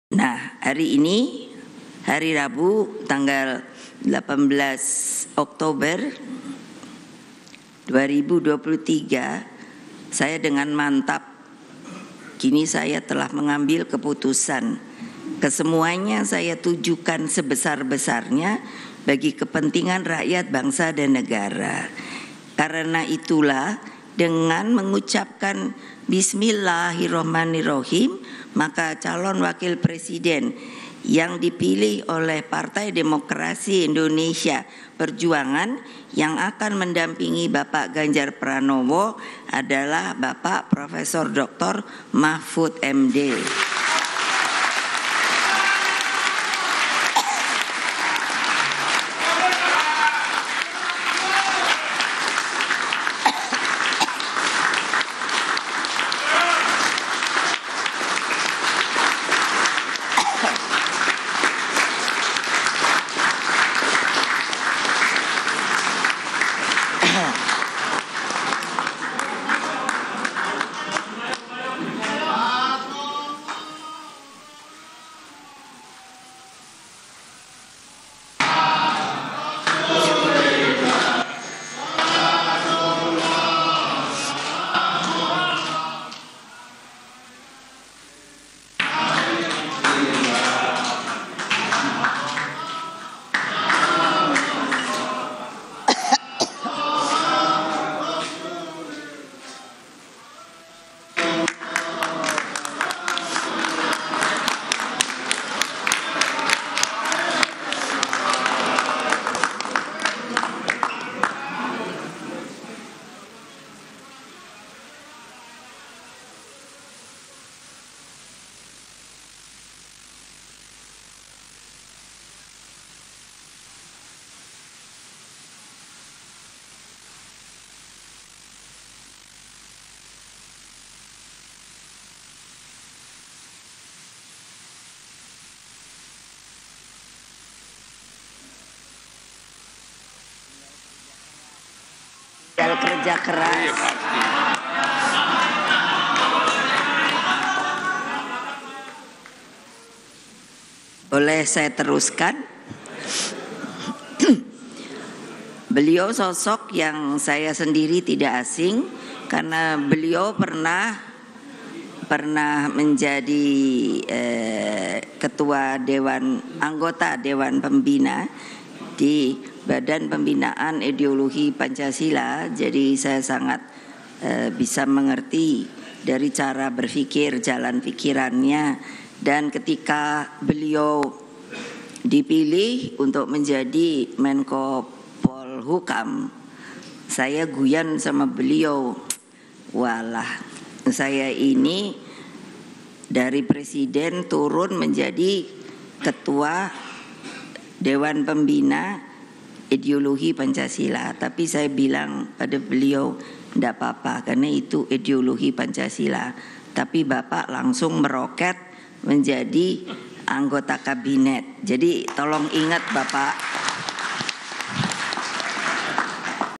Detik-detik Mahfud MD Dideklarasikan Jadi Bakal Cawapres Ganjar Pranowo
JAKARTA, KOMPAS.TV - Bakal calon presiden PDIP Ganjar Pranowo hari ini, 18 Oktober 2023 mengumumkan bakal cawapresnya.

Menko Polhukam Mahfud MD terpilih untuk dampingi Ganjar Pranowo sebagai calon wakil presiden di Pilpres 2024.